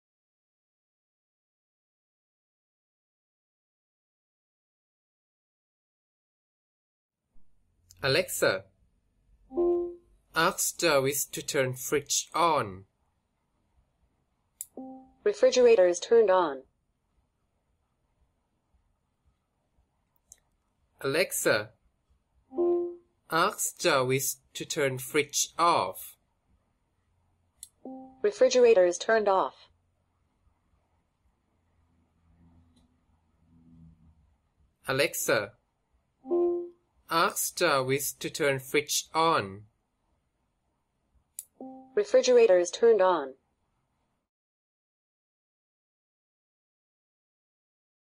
Amazon Echo Dot - Control Electric Appliance with Alexa
This video shows the demo of using Amazon Echo Dot with Alexa to control refrigerator by sending voice command.